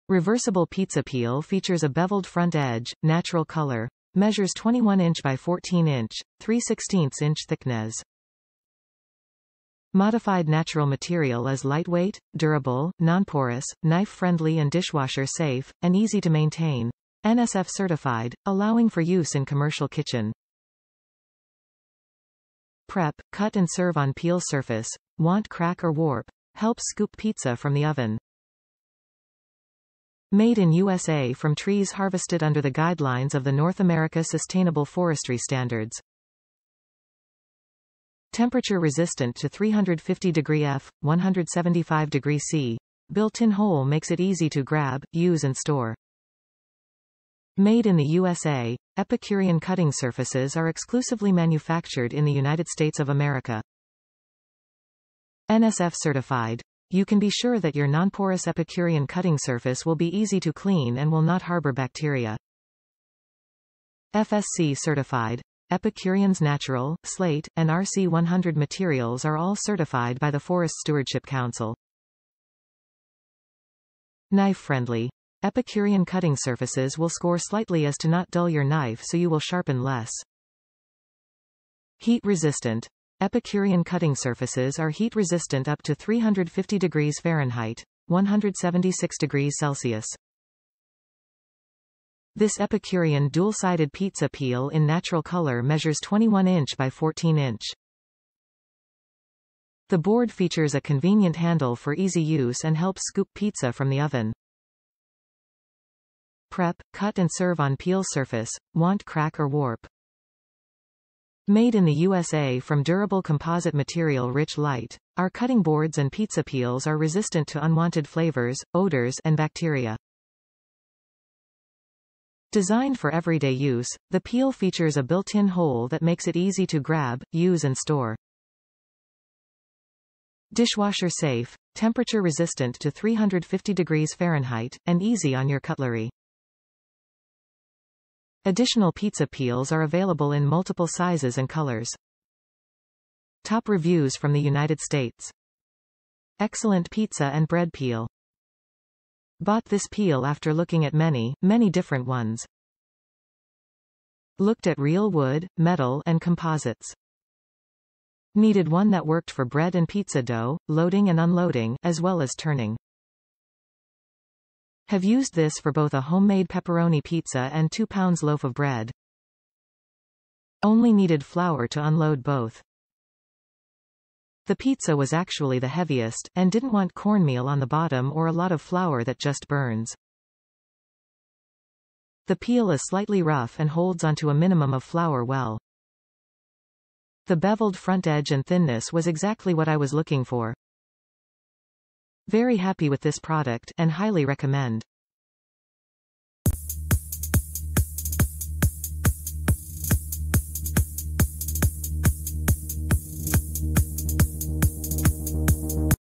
Epicurean Pizza Peel, 21-Inch by 14-Inch, Natural
Reversible pizza peel features a beveled front edge, natural color; measures 21-Inch by 14-inch; 3/16 inch thicknes
Modified natural material is lightweight, durable, nonporous, knife friendly and dishwasher safe, and easy to maintain; NSF certified, allowing for use in commercial kitchen
Prep, cut and serve on peel surface; won't crack or warp; helps scoop pizza from the oven
Made in USA from trees harvested under the guidelines of the North America sustainable Forestry standards
Temperature resistant to 350 Degree F/175 Degree C; built-in hole makes it easy to grab, use and store
Made in the USA: Epicurean cutting surfaces are exclusively manufactured in the United States of America. NSF Certified: you can be sure that your nonporous Epicurean cutting surface will be easy to clean and will not harbor bacteria. FSC Certified: Epicurean's Natural, Slate, and RC100 materials are all certified by the Forest Stewardship Council. Knife Friendly: Epicurean cutting surfaces will score slightly as to not dull your knife so you will sharpen less. Heat-Resistant: Epicurean cutting surfaces are heat-resistant up to 350 degrees F (176 degrees C). This Epicurean dual-sided pizza peel in natural color measures 21-inch by 14-inch. The board features a convenient handle for easy use and helps scoop pizza from the oven. Prep, cut and serve on peel surface; won't crack or warp. Made in the USA from durable composite material Rich lite, our cutting boards and pizza peels are resistant to unwanted flavors, odors, and bacteria. Designed for everyday use, the peel features a built-in hole that makes it easy to grab, use and store. Dishwasher safe, temperature resistant to 350 degrees Fahrenheit, and easy on your cutlery. Additional pizza peels are available in multiple sizes and colors.

Excellent pizza and bread peel!
Bought this peel after looking at many, many different ones. Looked at real wood, metal, and composites. Needed one that worked for bread and pizza dough, loading and unloading, as well as turning. Have used this for both a homemade pepperoni pizza and 2 lbs loaf of bread. Only needed flour to unload both. The pizza was actually the heaviest, and didn’t want cornmeal on the bottom or a lot of flour that just burns. The peel is slightly rough and holds onto a minimum of flour well. The beveled front edge and thinness was exactly what I was looking for. Very happy with this product, and highly recommend.